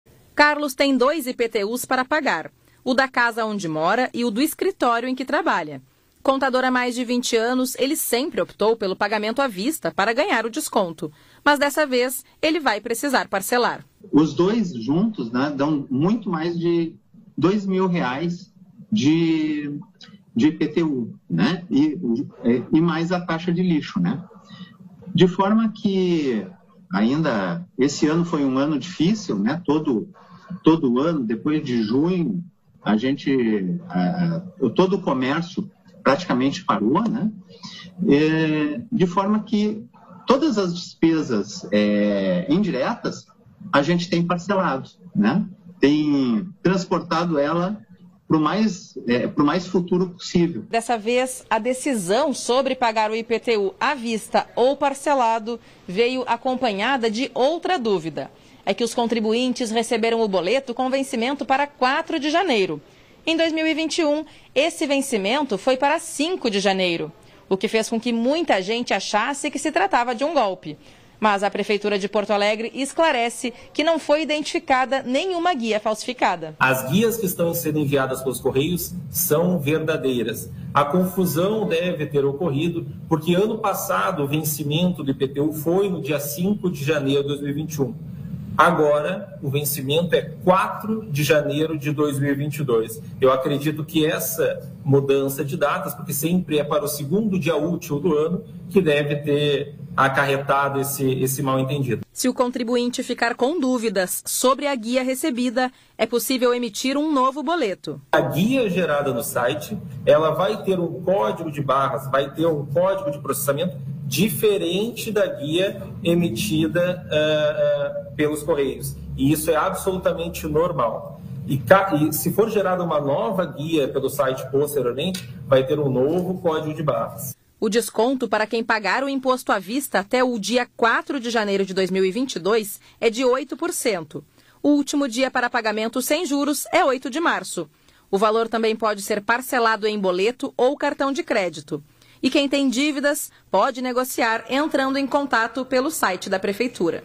IPTU: desconto vai até 4 de janeiro na capital
O ano ainda não terminou e já é preciso pensar nas contas a pagar em 2022. Entre elas, o IPTU. A prefeitura de Porto Alegre já está emitindo os boletos, com desconto para pagamento antecipado.